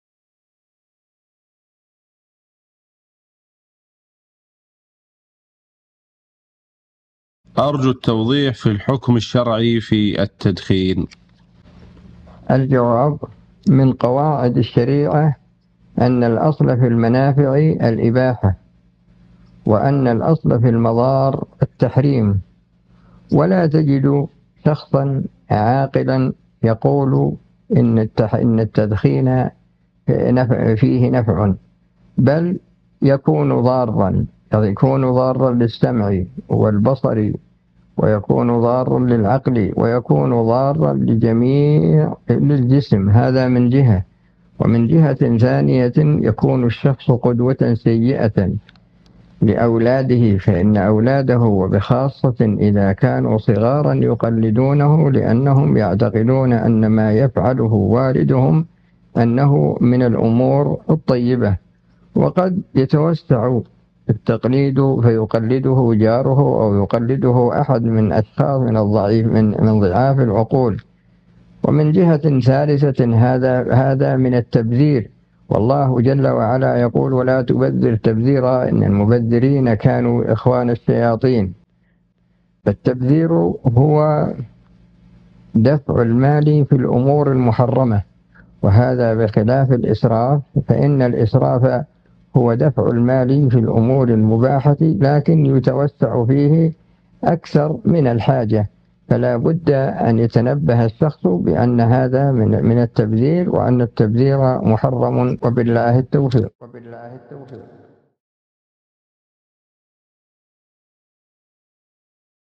🚬 EST-T'IL HARÂM DE FUMER 🚬?? Sheikh ABD-ALLAH AL-GOUDAYYÂN
Est-il harâm de fumer ?

L'érudit `Abd Allâh al-Ghoudayyân رحمه الله